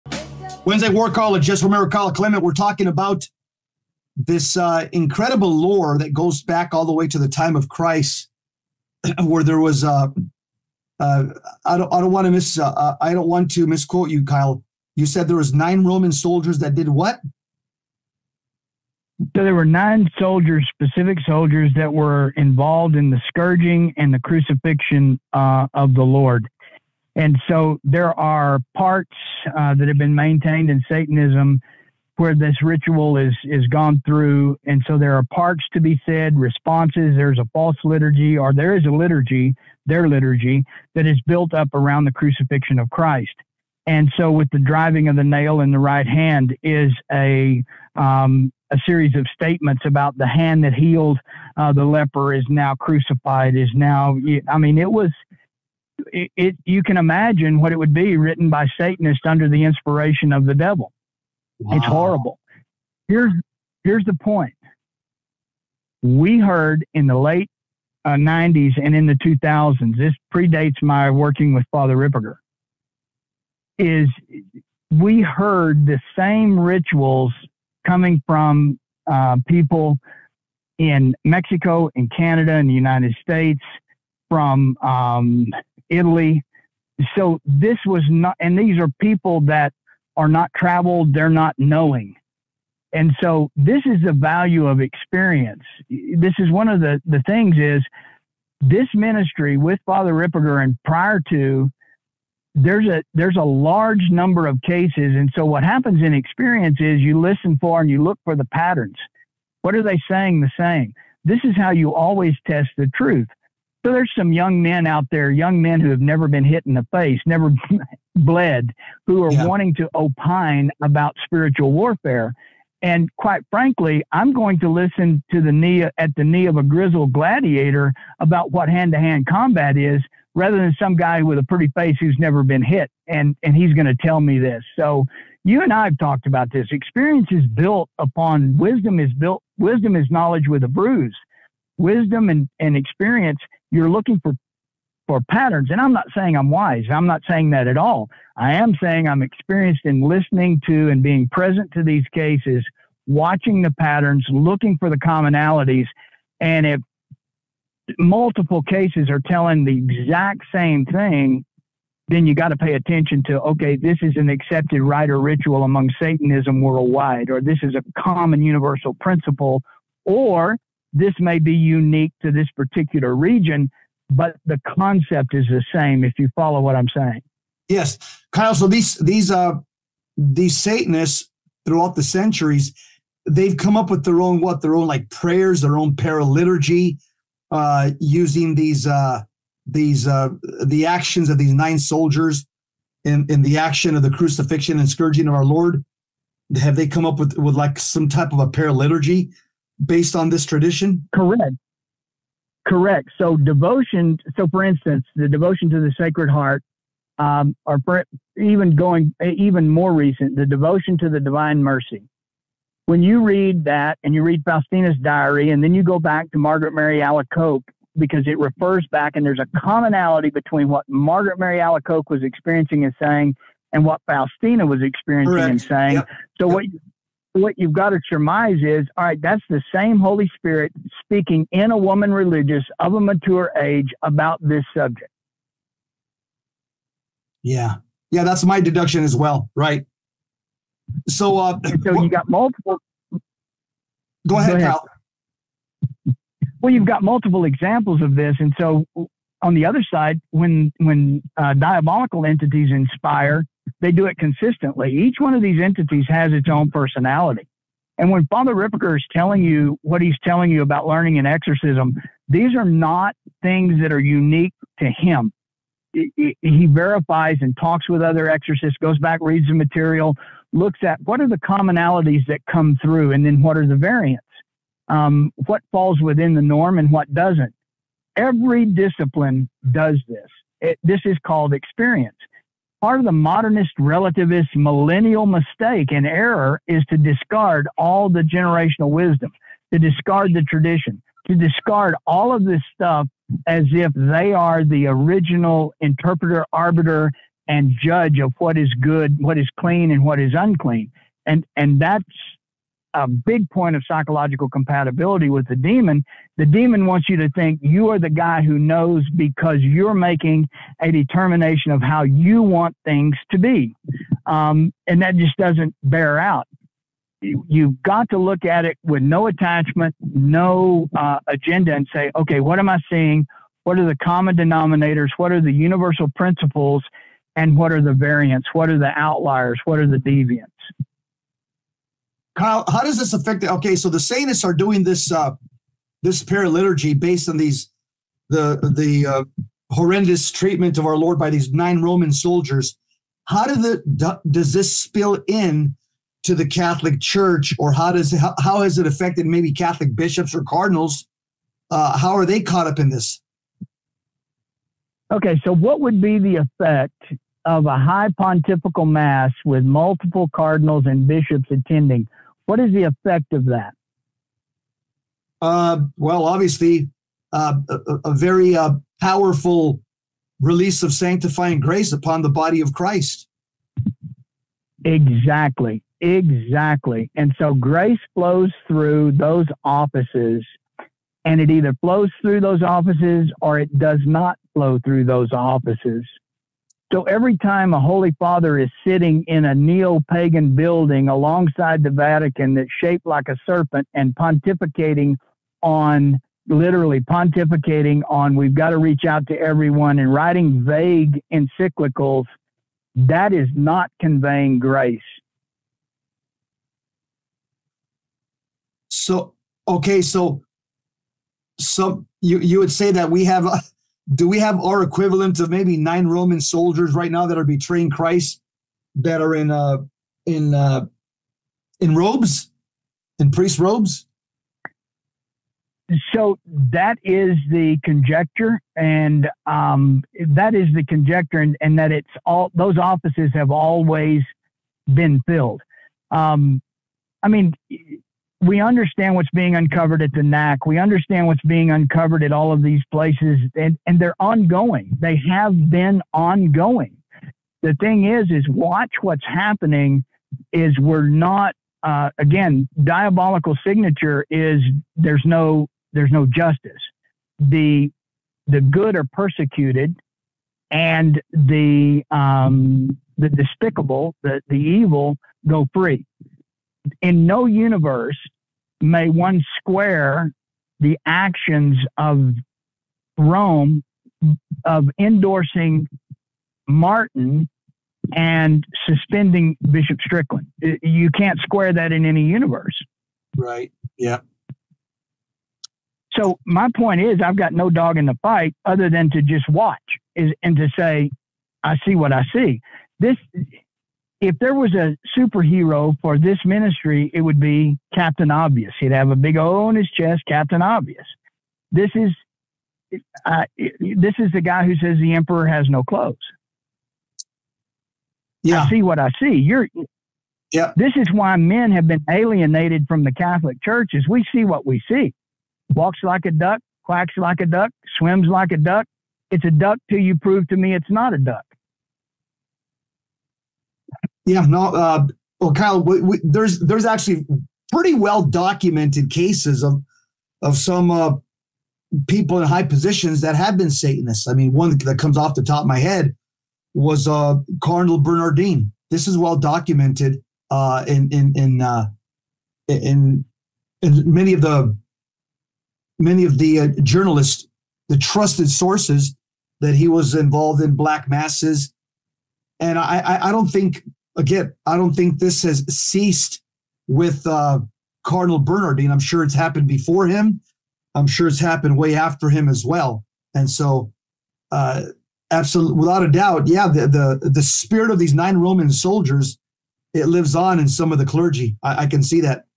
9 Demons of the Roman Soldiers in Christ Passion
Make sure to watch the entire show with Jesse Romero and Dr Dan Schneider
We have a FREE way for you to listen to the show live.
or
Keep us on the air with your prayers and donations at VMPR.org